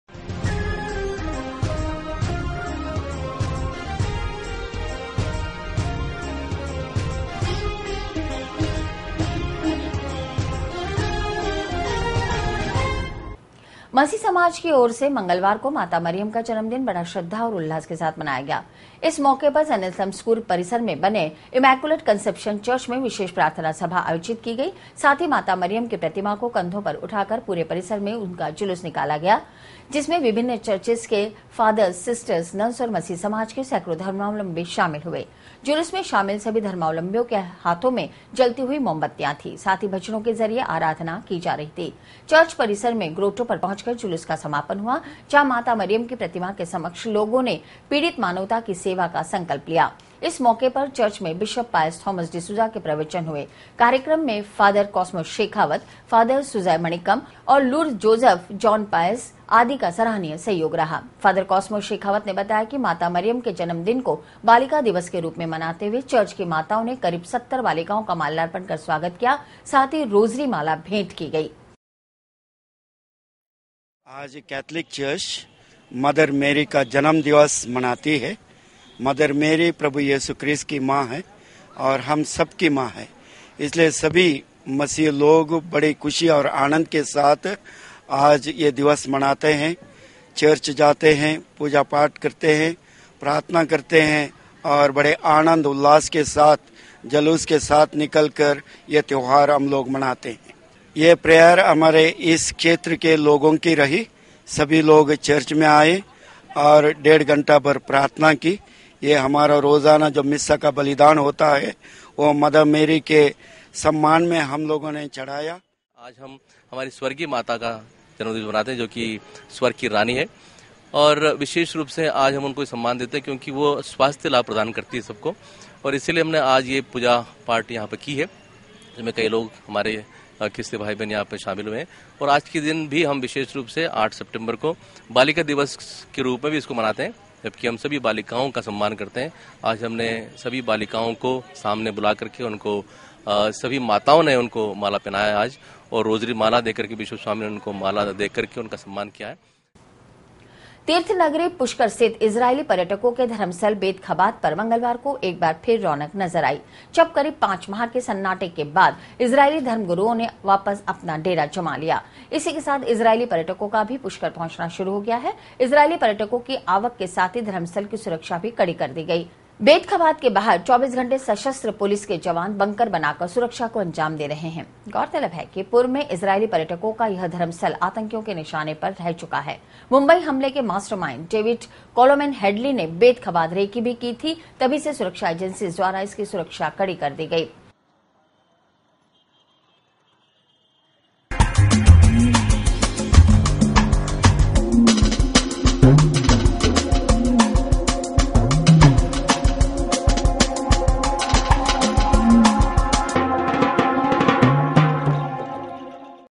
मदर मैरी का जन्मदिन मनाया मसीह समाज ने।
मदर मैरी का जन्मदिन मनाया मसीह समाज ने बड़े उल्लास के साथ। वहीं पुष्कर के बेद खबाद में ईजराईलियों के अगमन से फिर से लौटी रौनक।